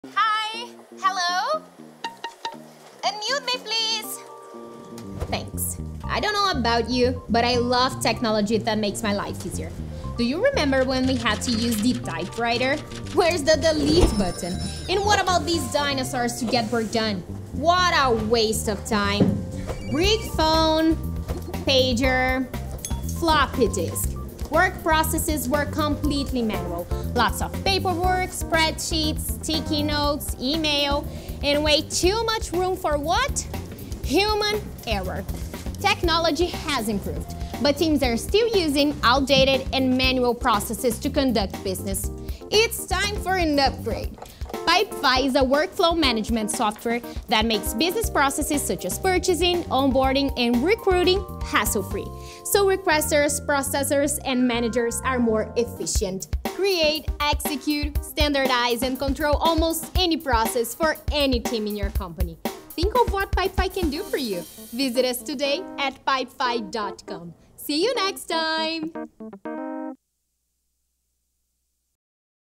About Pipefy - Exploring the Benefits of Low-code BPA Software
Pipefy is the workflow management software that makes business processes—such as purchasing, onboarding, and recruiting—hassle-free, so requesters, processors, and managers are more efficient. Through automated workflows, Pipefy enhances speed, increases visibility, and delivers higher quality outcomes without the need of IT implementation.